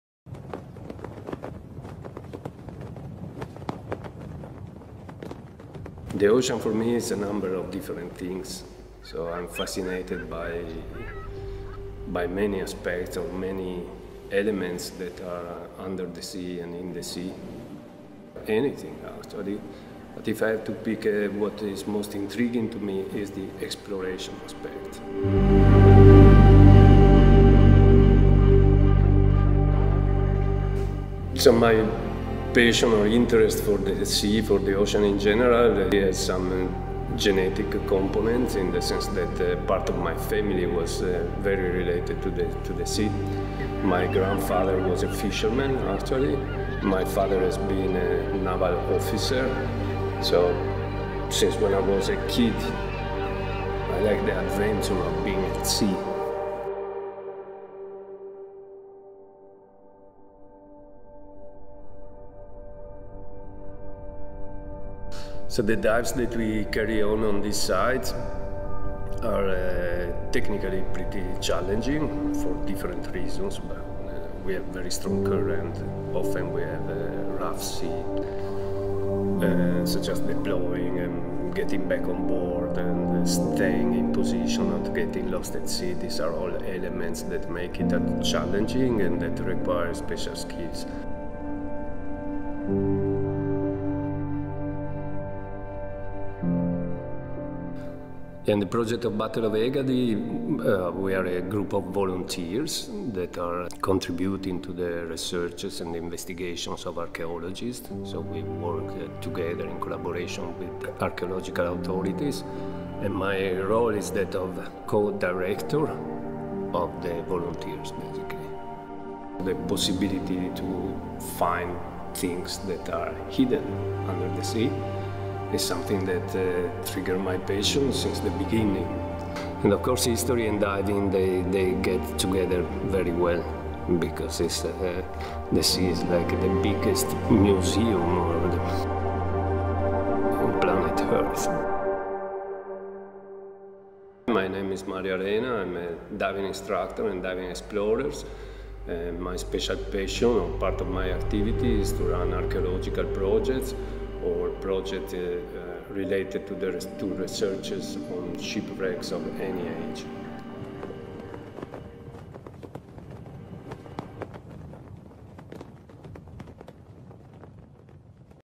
Mario Arena - Diving explorer & DAN member
The sea “is like the biggest museum on planet earth” in the eyes of diving instructor and explorer Mario Arena. His two passions, for the sea and for naval history, run deep within his veins. His mother was a history teacher and his father was a naval engineer. Today, Mario forms part of a team of global explorers who are helping with the retrieval of priceless artefacts from the underwater archaeological site where hundreds of ships sank during the 241 BC Battle of the Aegates Islands.